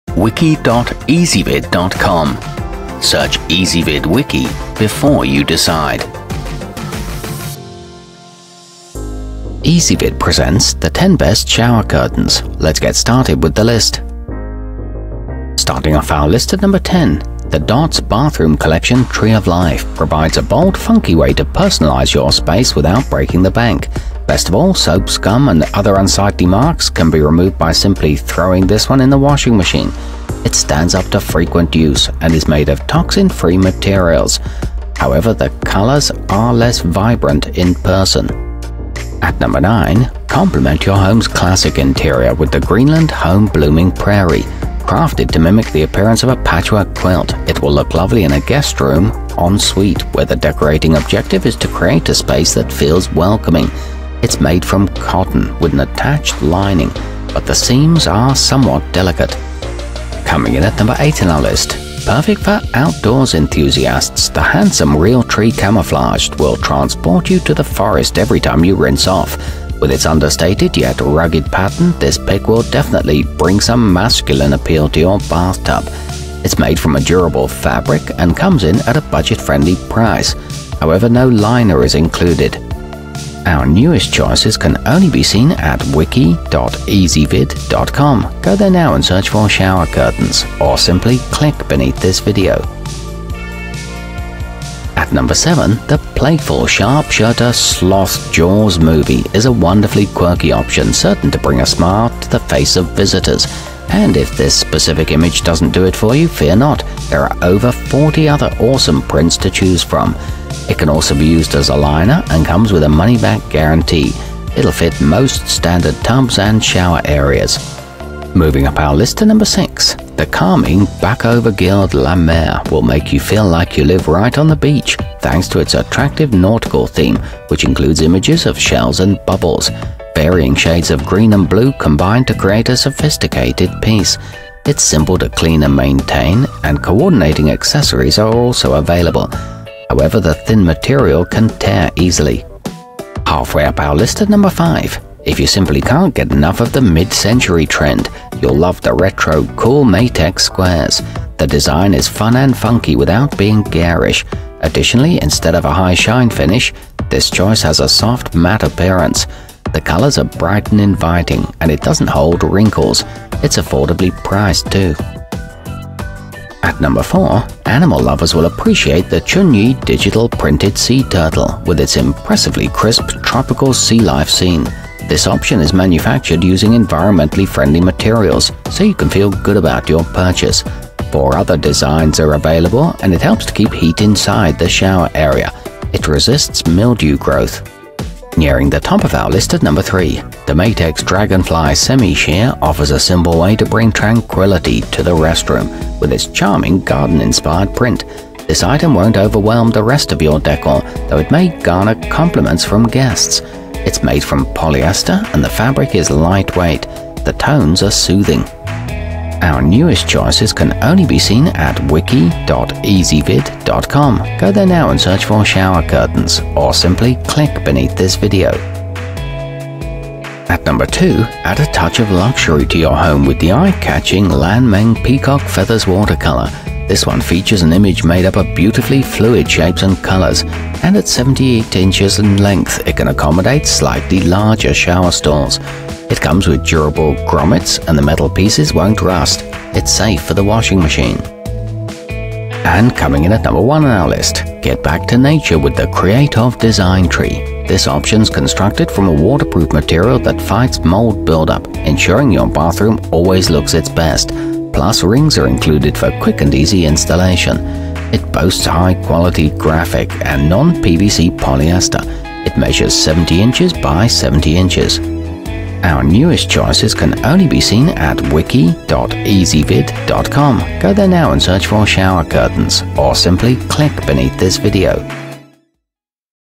10 Best Shower Curtains 2017
Our complete review, including our selection for the year's best shower curtain, is exclusively available on Ezvid Wiki.

Shower curtains included in this wiki include the chunyi digital printed sea turtle, lanmeng peacock feathers watercolor, maytex squares, bacova guild la mer, realtree camouflaged, greenland home blooming prairie, maytex dragonfly semi sheer, creatov design tree, dotz bathroom collection tree of life, and sharp shirter sloth jaws movie.